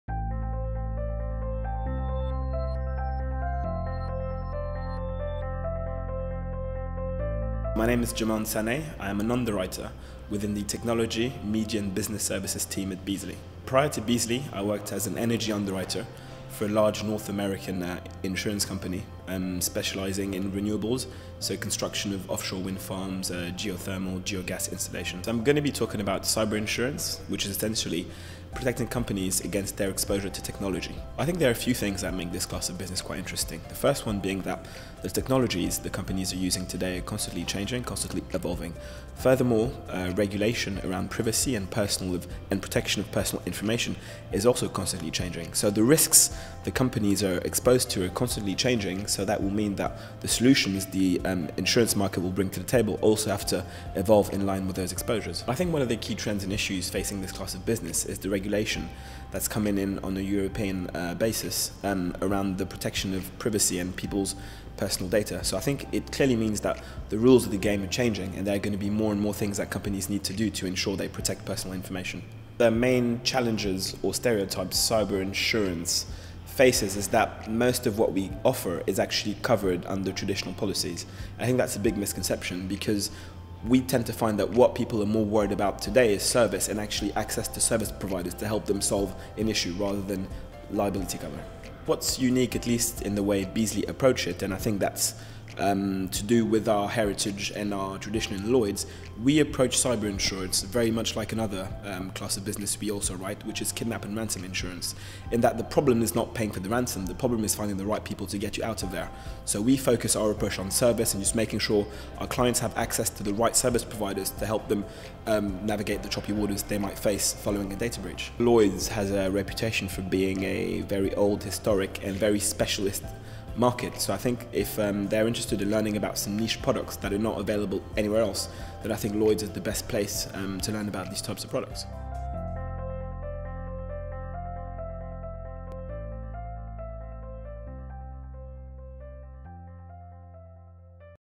Lloyd's Global Development Centre - Jimaan Sane on Cyber Risk
Jimaan Sane, Technology Underwriter at Beazley, gives his insights into Cyber Risk and addresses some of the stereotypes and issues and challenges for this class of business.

He also introduces the topics and themes he will be discussing as part of the Lloyd's Global Development Centre programme, showcasing the knowledge and expertise in the Lloyd's market.

to find out more about Lloyd's and the Global Development Centre programmes.